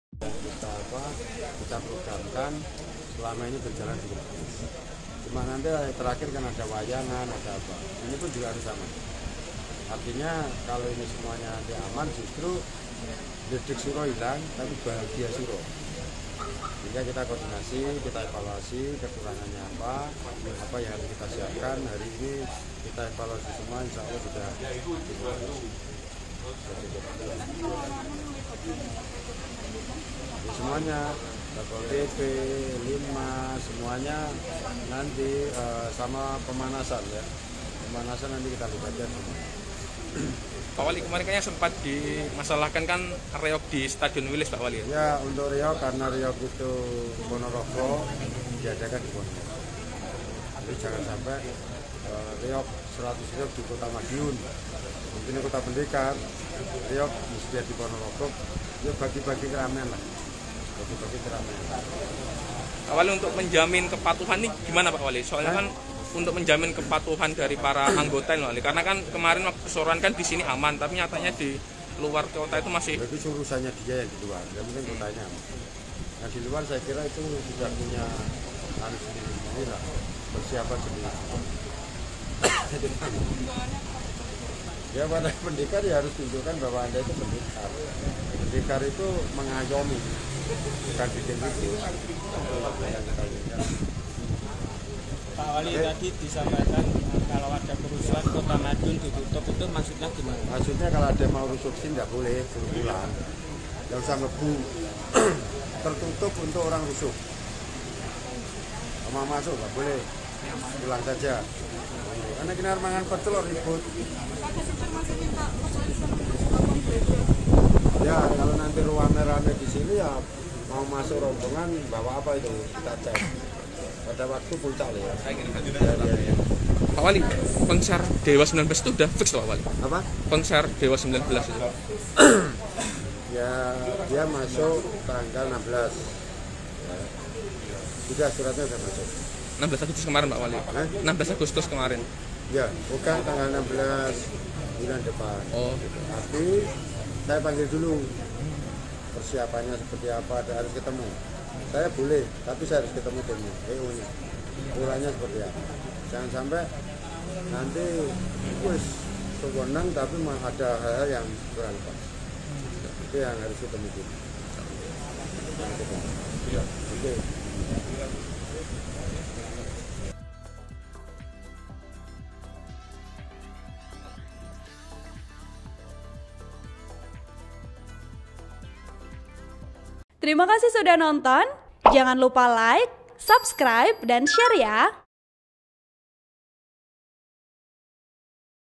Wali Kota Madiun Tak Melarang Acara Peringatan 100 Tahun Terate Emas, Asalkan Jaga Kondusivitas
TRIBUNJATIM.COM, MADIUN - Wali Kota Madiun, Maidi tak melarang resepsi peringatan 100 tahun organisasi pencak silat Persaudaraan SH Terate atau Terate Emas digelar di Kota Madiun tanggal 2 September, asalkan berjalan aman dan kondusif.

Acara yang menjadi perhatian Maidi adalah tontonan yang menarik masyarakat umum seperti pagelaran wayang kulit dan pertunjukan Reog di Stadion Wilis.

Dari hasil beberapa kali rapat koordinasi, pagelaran Reog telah diputuskan akan digelar di Ponorogo.

"Peringatan Suroan selama ini berjalan cukup kondusif, cuma terakhir nanti kan ada wayangan ini juga harus berjalan dengan aman," kata Maidi ditemui di GCIO Kota Madiun, Senin (22/8/2022).

Artikel ini telah tayang di Surya.co.id dengan judul Wali Kota Madiun Ajak Pendekar Jaga Kondusivitas saat Peringatan 100 Tahun Terate Emas,
Penulis: Sofyan Arif Candra Sakti | Editor: Titis Jati Permata